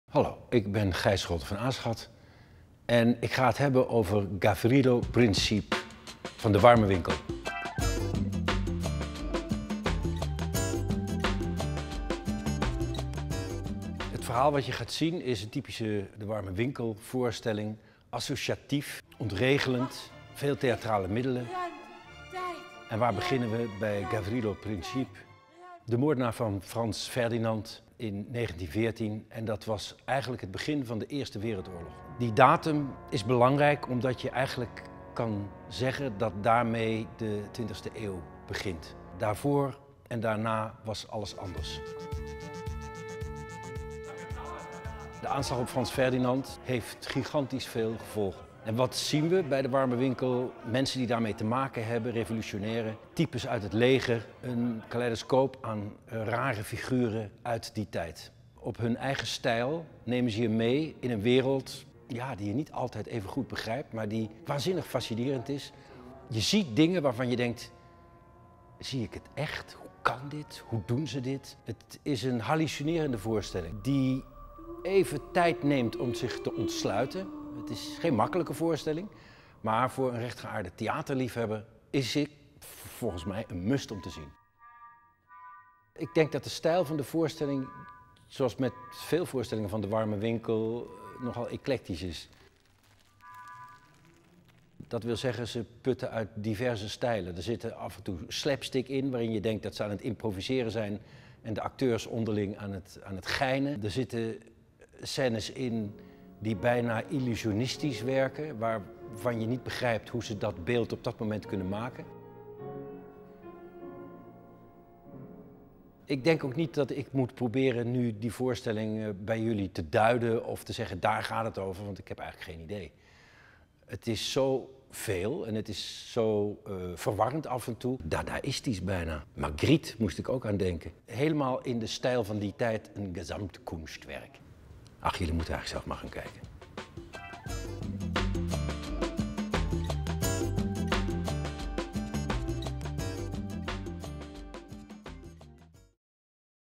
Gijs Scholten van Aschat over Gavrilo Princip
Gijs Scholten van Aschat bespreekt de voorstelling Gavrilo Princip van De Warme Winkel. Gavrilo Princip is van 10 t/m 12 september te zien tijdens het Nederlands Theater Festival in Stadsschouwburg Amsterdam, kaarten zijn te koop